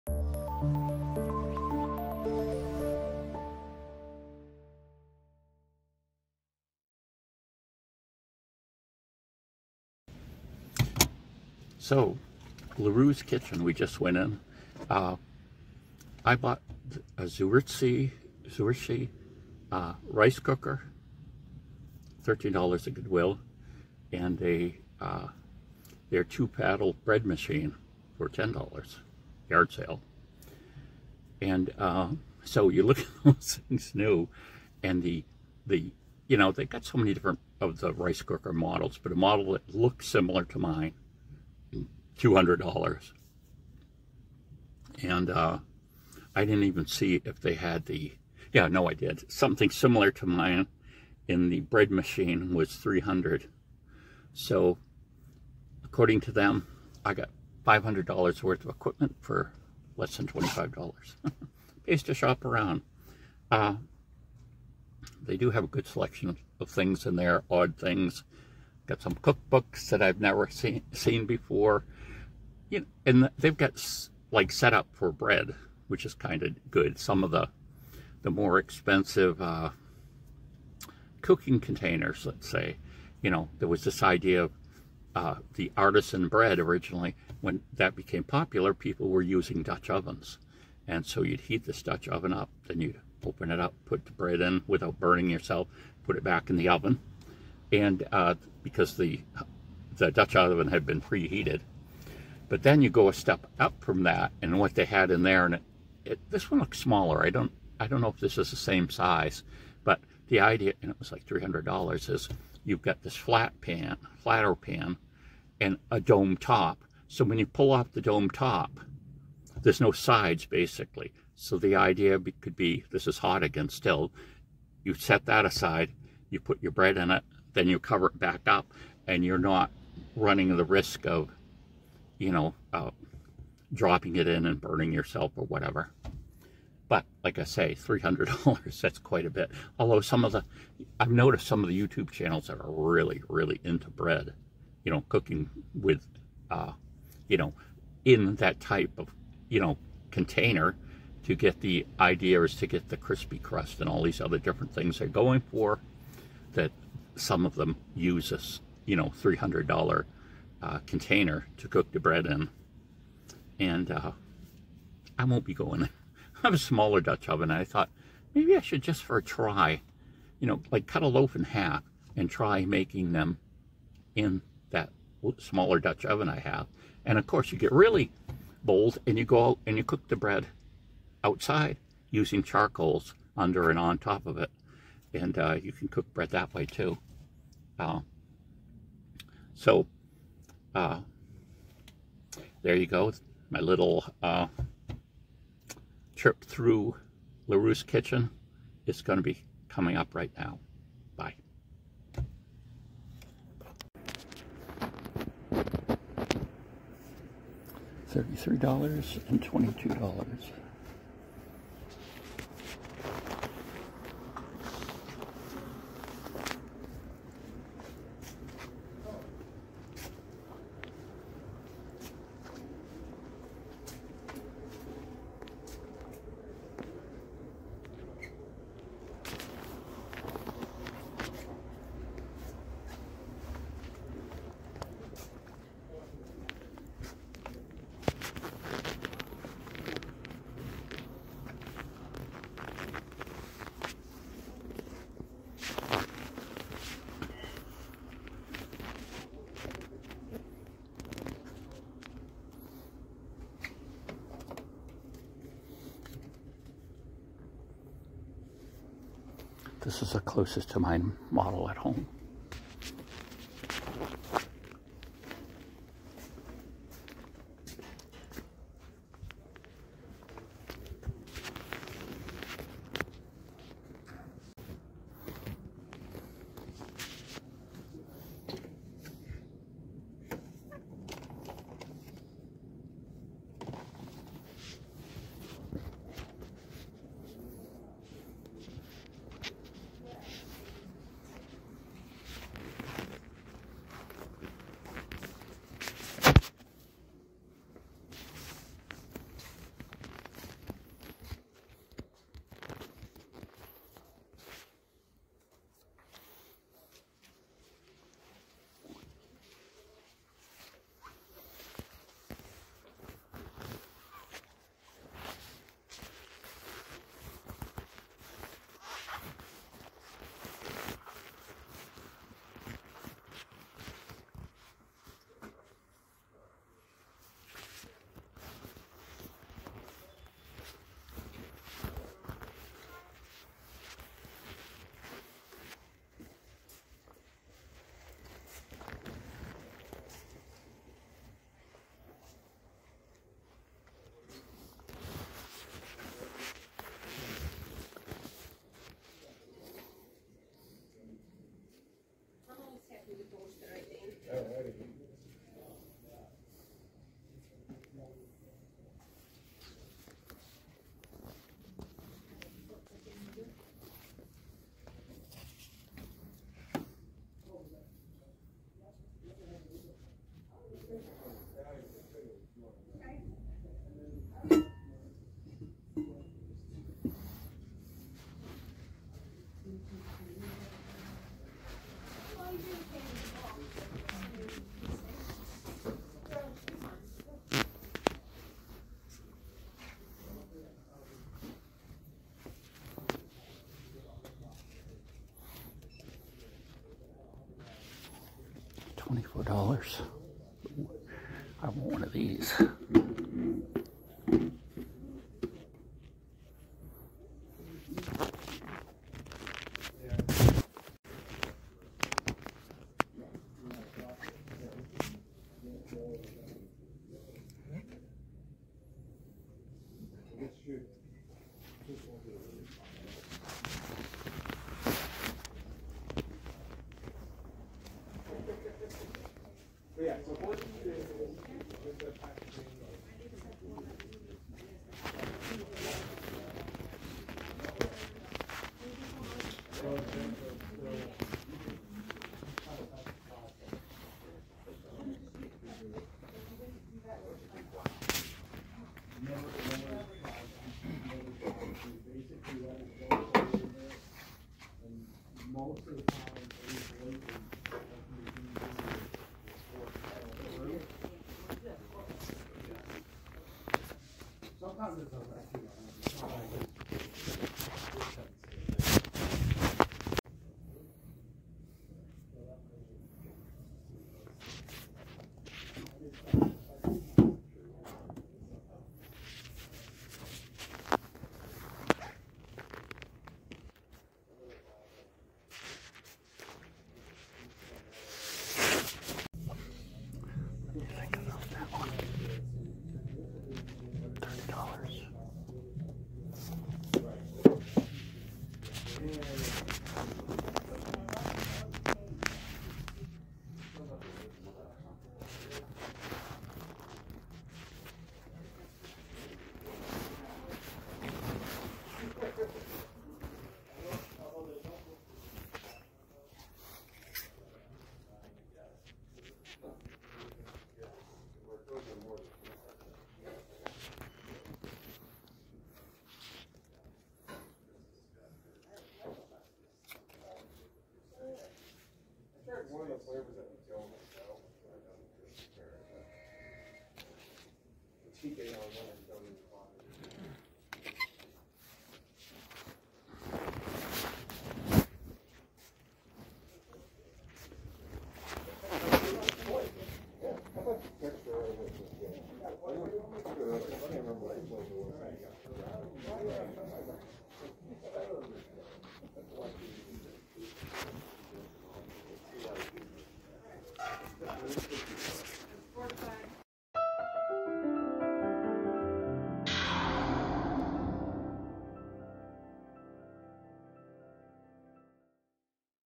LeRoux Kitchen Store
Don't miss this LeRoux store visit! I show you some of the coolest and weirdest items they have from 3:26 in the video. First, I talk about Goodwill purchases of similar items.